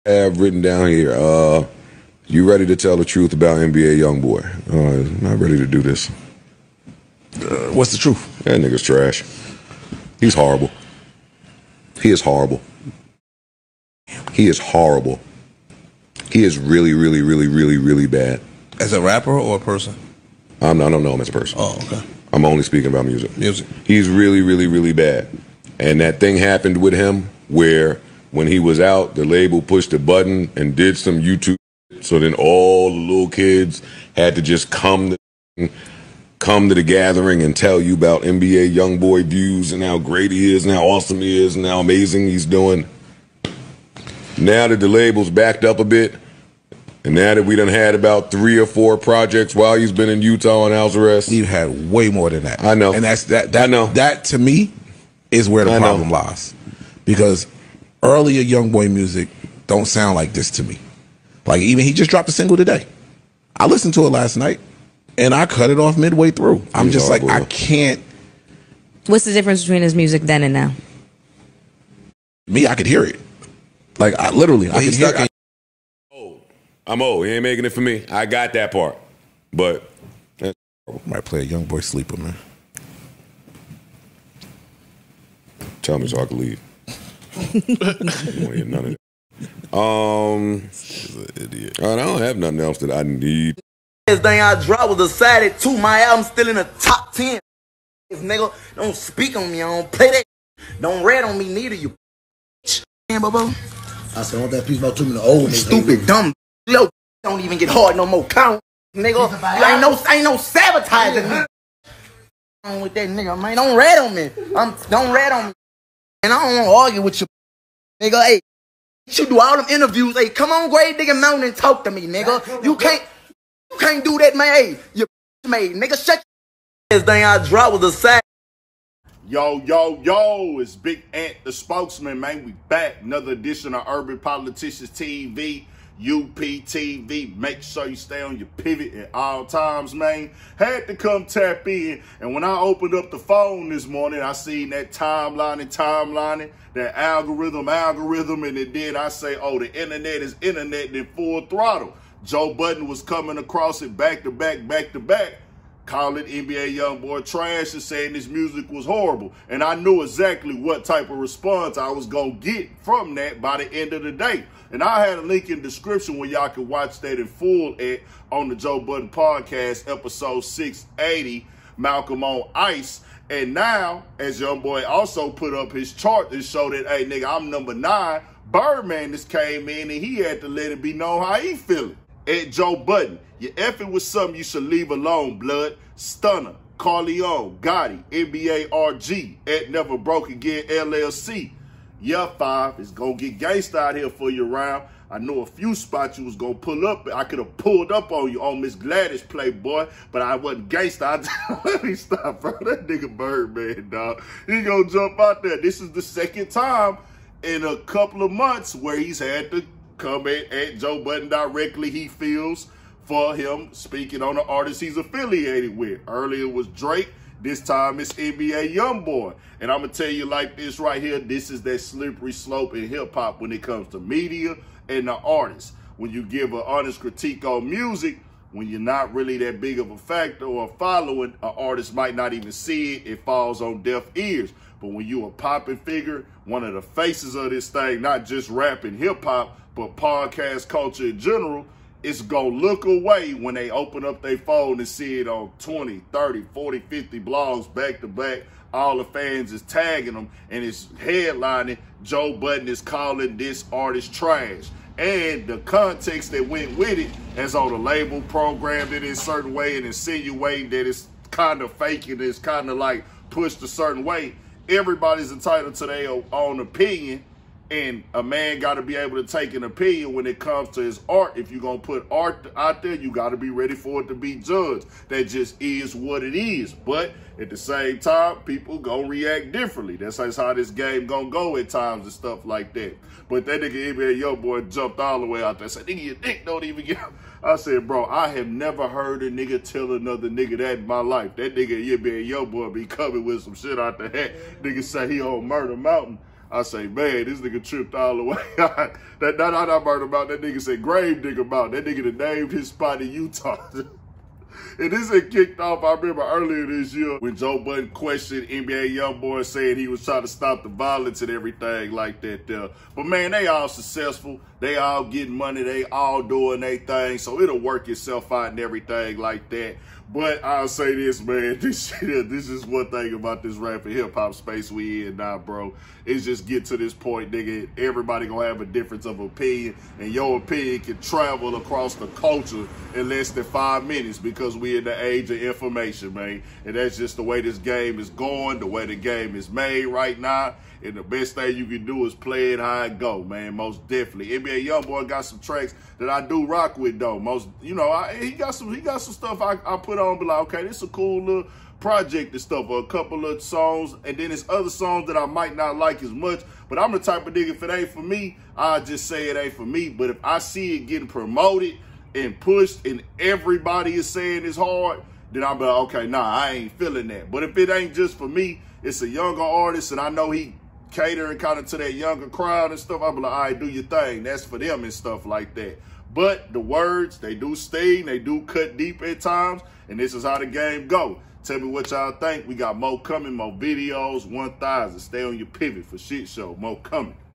Birdman REACTS To Joe Budden Calling NBA Youngboy TRASH & SENDS WARNING “YOU F**KIN WITH SOME…
Birdman Reacts to joe budden coments on NBA Youngboy music.